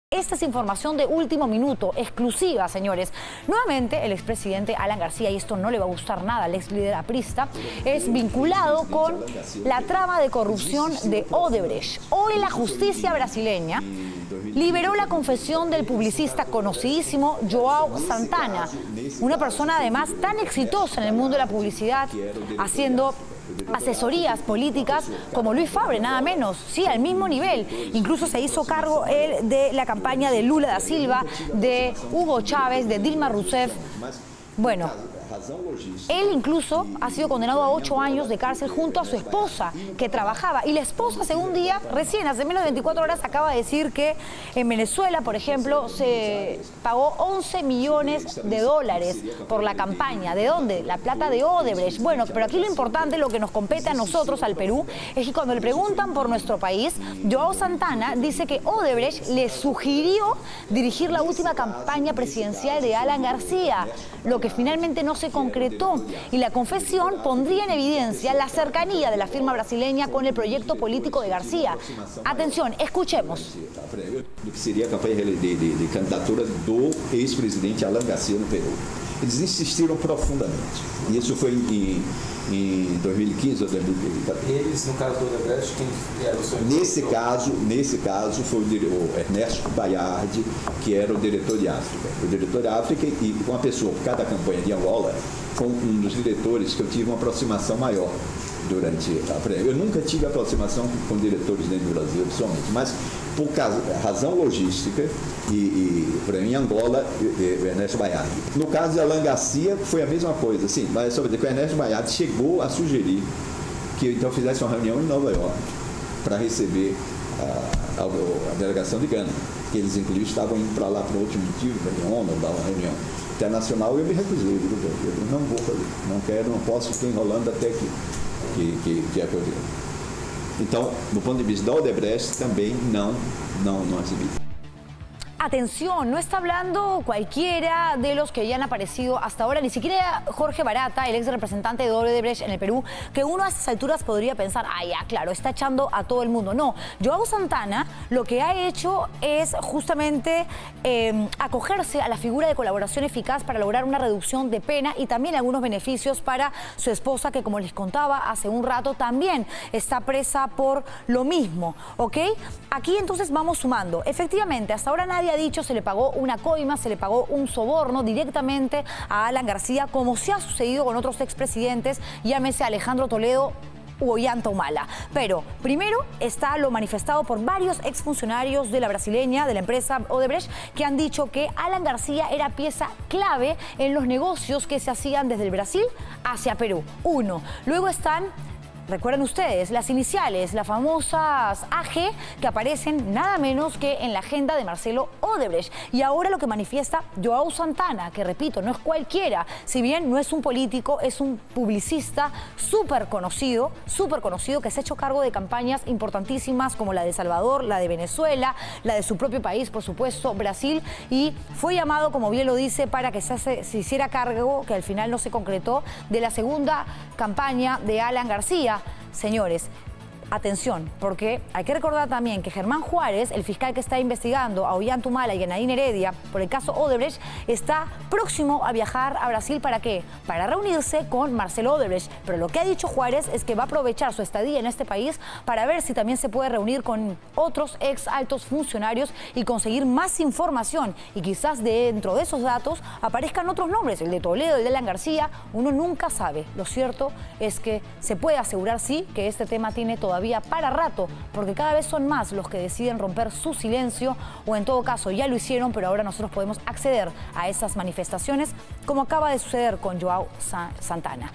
Alan García: Empresario Joao Santana dice que Odebrecht le pidió asesorarlo
Nuevamente Alan García se ve vinculado a los casos de corrupción de Odebrecht. Esta vez se supo que la empresa le pidió al publicista Joao Santana que asesore al líder aprista.

El expresidente Alan García nuevamente es vinculado a la trama de corrupción de la empresa Odebrecht. La justicia brasileña liberó este viernes la confesión del publicista Joao Santana, quien señaló que la mencionada empresa le pidió dirigir la campaña presidencial del líder aprista, algo a lo que finalmente no accedió.